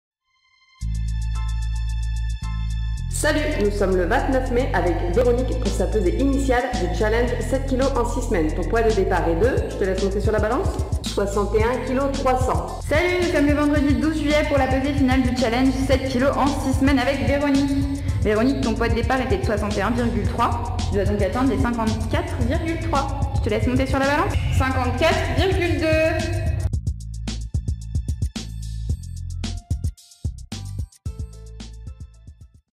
Transformation physique rapide Lyon
Véronique a perdu 7,100 kgs en 6 semaines à la Training Academy